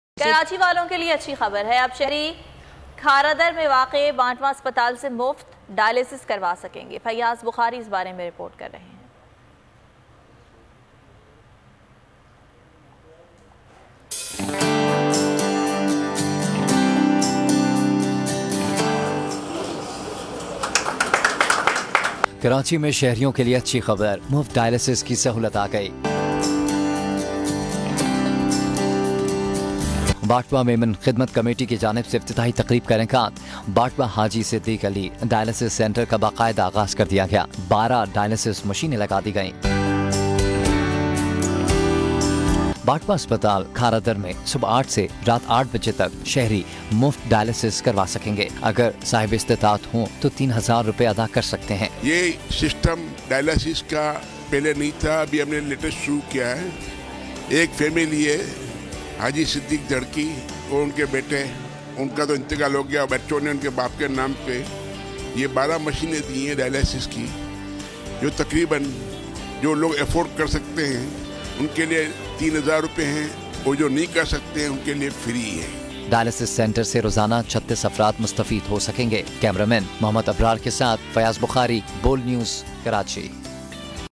افتتاح بانٹوا صدیق حاجی علی محمد دھڑکی Dialysis Word بانٹوا ہسپتال کھارادر کراچی مورخہ12 اکتوبر 2022
BNN Production Presents